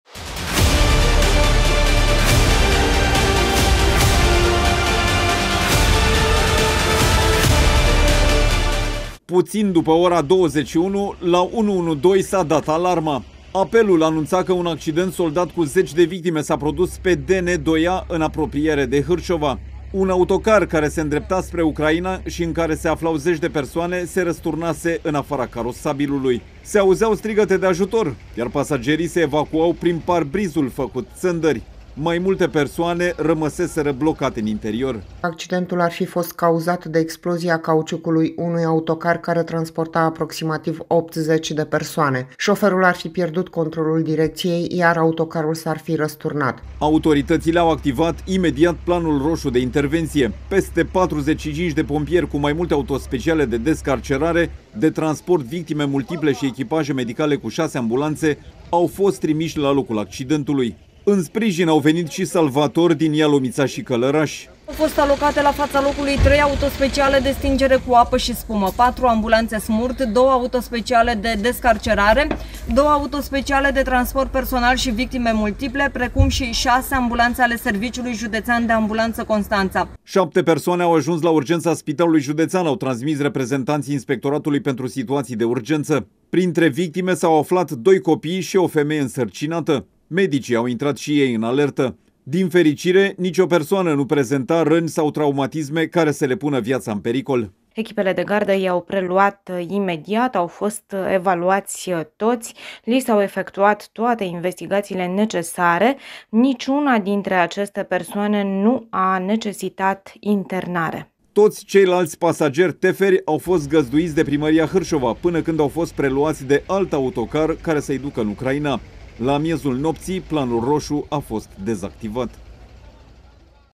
S-a activat planul roșu de intervenție în Constanța
A fost activat Planul Roșu de Intevenție la Constanța, noaptea trecută, după ce un autocar cu peste 80 de ucraineni s-a răsturnat în apropiere de Hârșova. Mai multe persoane au rămas încarcerate în interior. Pentru a salva victimele, la fața locului au fost trimiși zeci de pompieri și paramedici, inclusiv din județele învecinate.